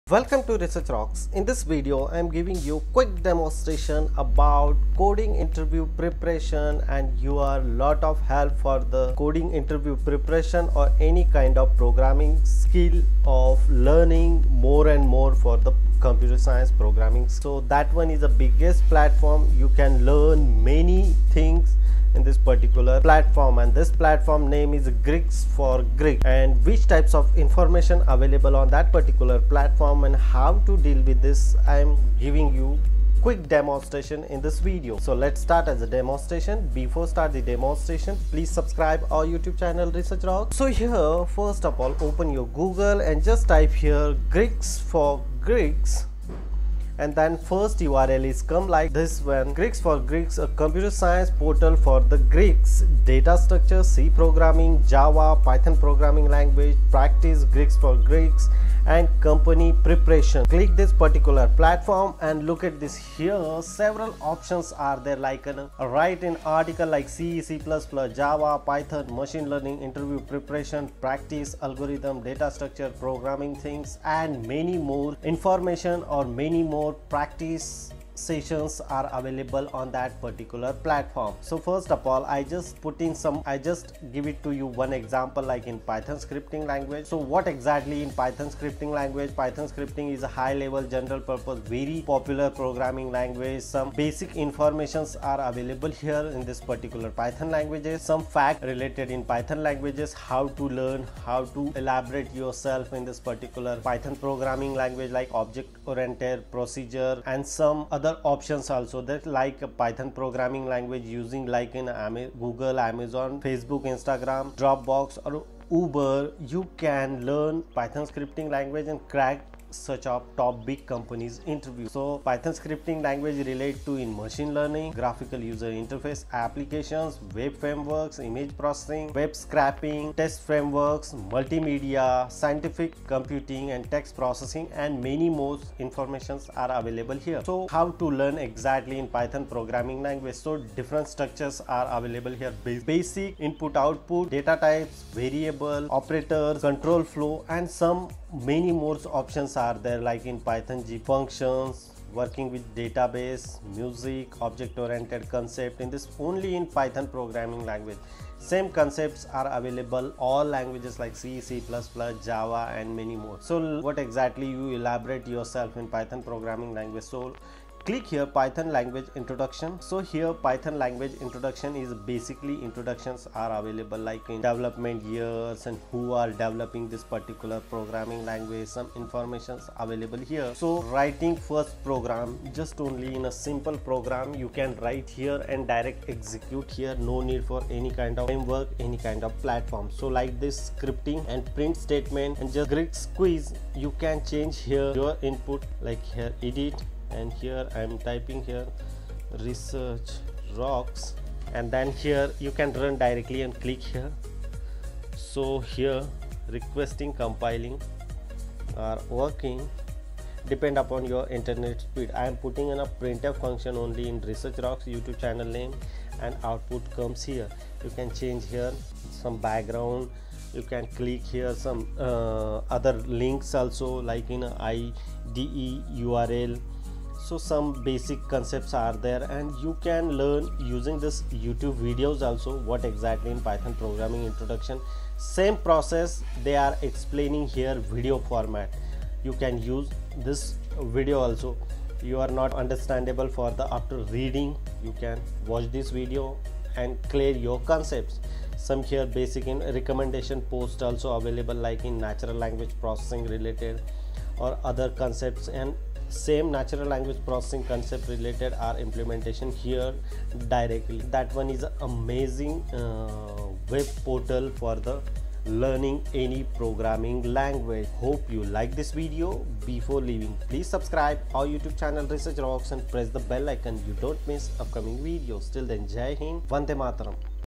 How to Use GeeksForGeeks for Coding Practice | Master Programming on GeeksForGeeks
In this video, discover how GeeksForGeeks can become your ultimate coding companion. Learn how to navigate through its vast collection of coding problems, tutorials, and resources to sharpen your programming skills. Whether you're a beginner or an experienced developer, GeeksForGeeks offers an extensive library to help you practice coding and ace your next technical interview. Watch now to unlock the secrets to mastering programming with GeeksForGeeks!